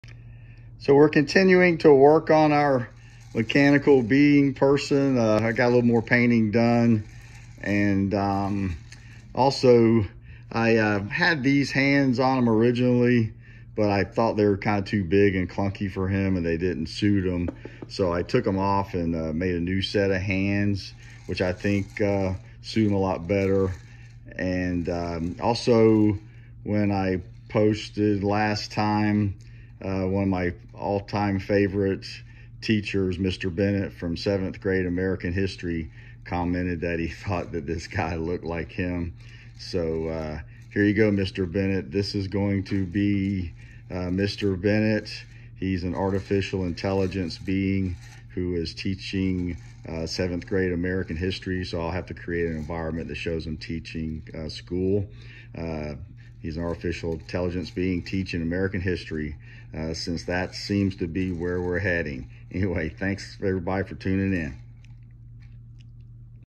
“Mr Bennett, Artificial Intelligence Mechanical Being Teaches Seventh Grade American History”
This story of Mr Bennett, Artificial Intelligence Mechanical Being Teaching Seventh Grade American History is inspired by the real Mr Bennett, one of my all time favorite teachers! This will be a Narrative Sculpture. I am going to create an environment that shows this AI Being teaching Seventh Grade American History. Figure is sculpted from Polymer Clay. Environment will be realistic and constructed using mixed media.